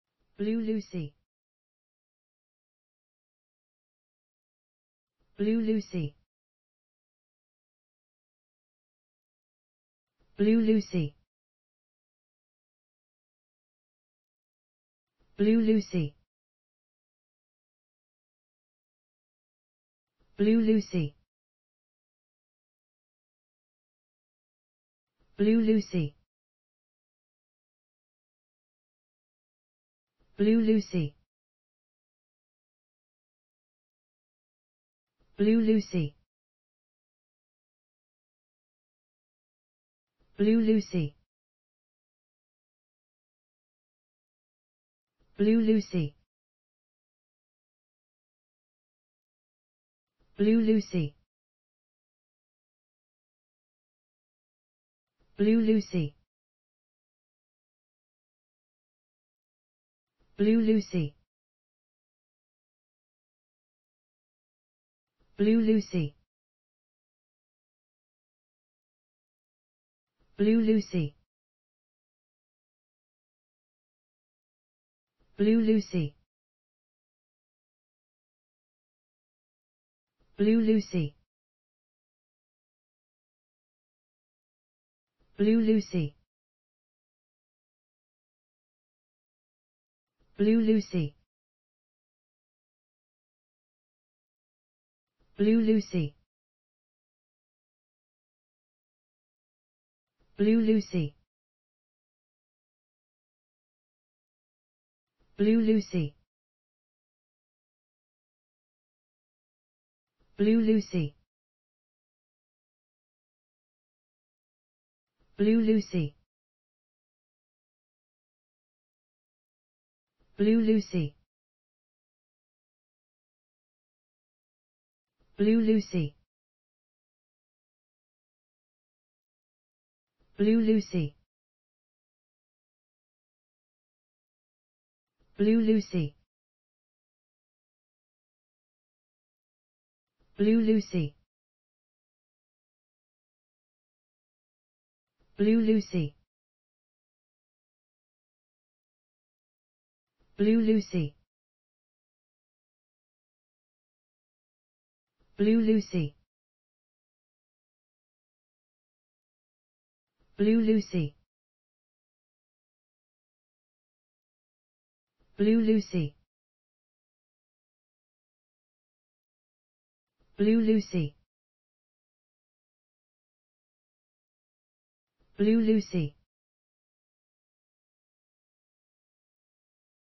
Blue Lucy. How to pronounce the english word Blue Lucy .Start with B. Learn from me.
Follow me!Learn from me how to pronounce the english word Blue Lucy.This video shows you how to pronounce the English word Blue Lucy .